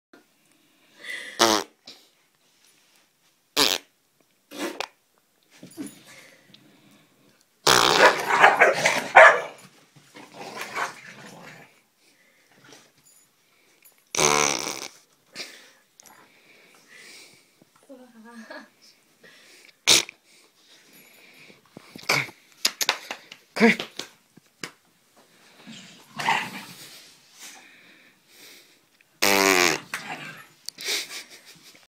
crazy farting dog
dog farts then chases tail